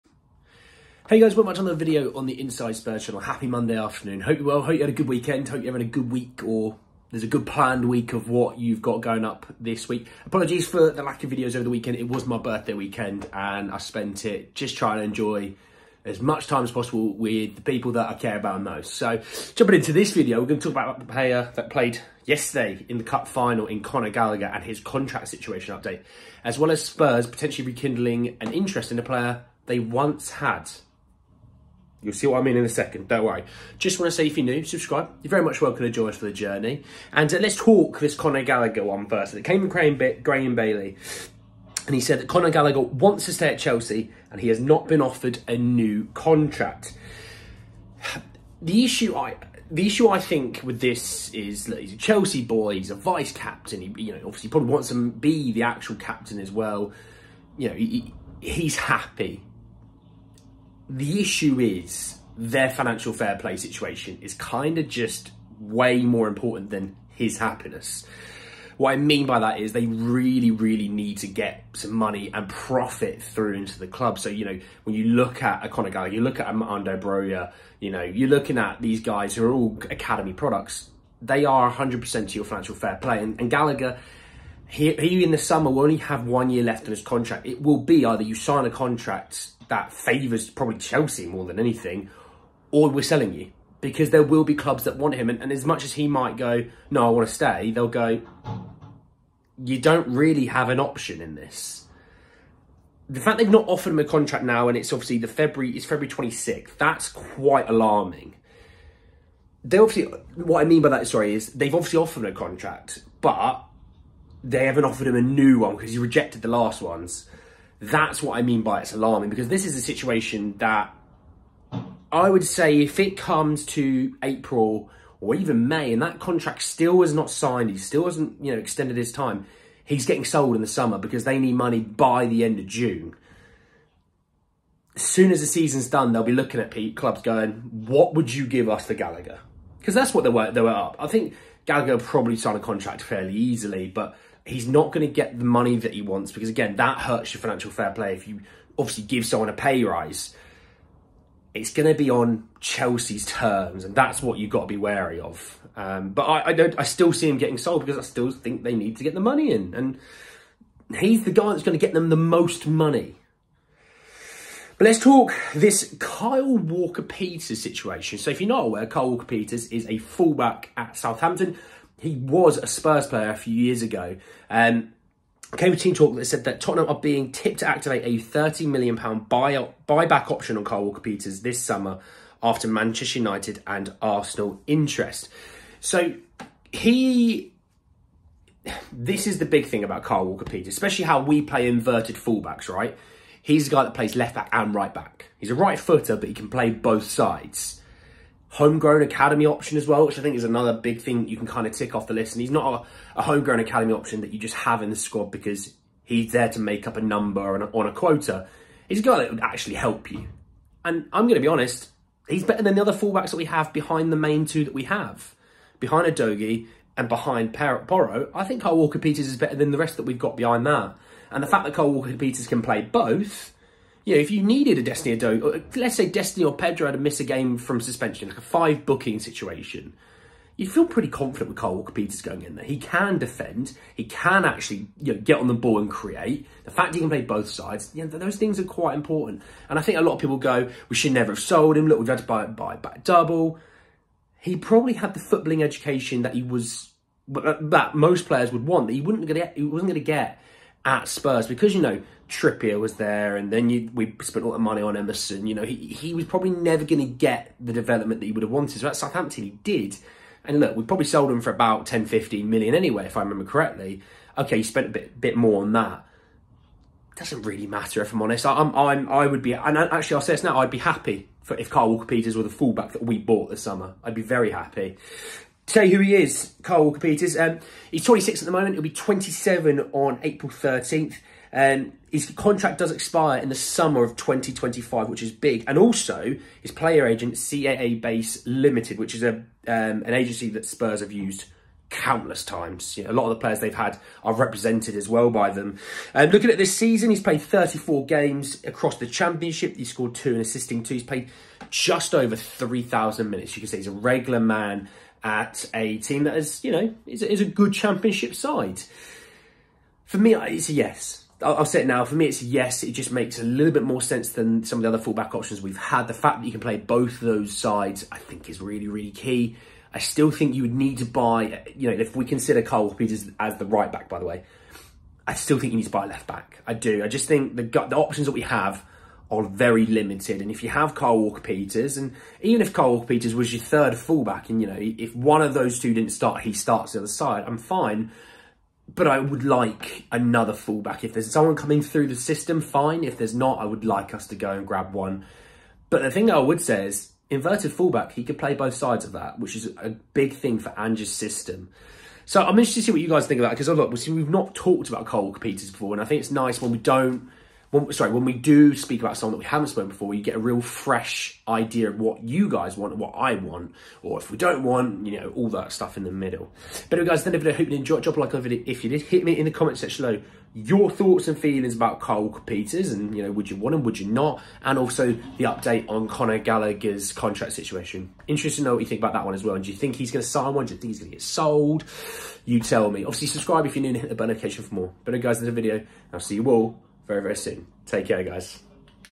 GALLAGHER NOT BEEN OFFERED CHELSEA NEW DEAL? SPURS WANT KYLE WALKER-PETERS? SPURS TRANSFER NEWS
We react to the report that conor gallagher hasn’t been offered a new chelsea deal, as well as a report linking spurs with former player Kyle Walker peters.

X: @insidespurstv
X: @andrew_lavy